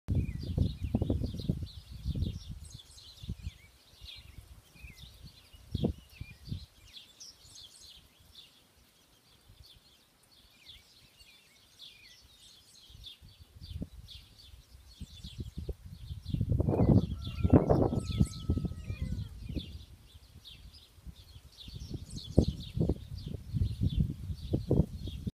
Kitty bird hunting
Our farm cat Mrs. onslow is watching the birds that get in the chicken coop.